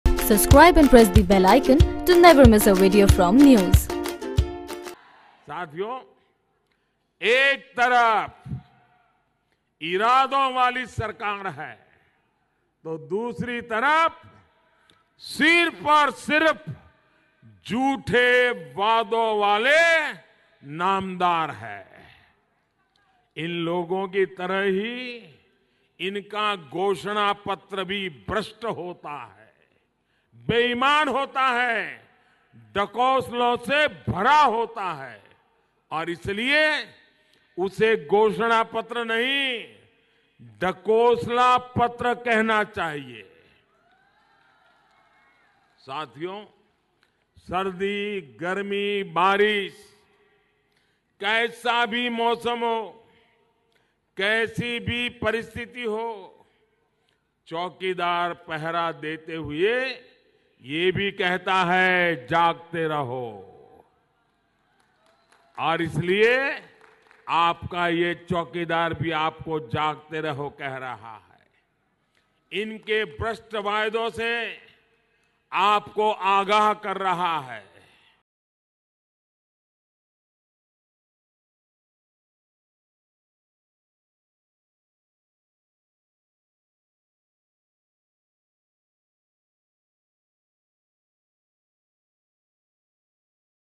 Manifesto of lies, Hypocrisy: PM Modi on Congress Manifesto
PM Modi in Arunachal Pradesh: The 2019 election, said the Prime Minister, "is an election between truth and lies, between those who deliver and those who make false promises, between those you can trust and those who are corrupt."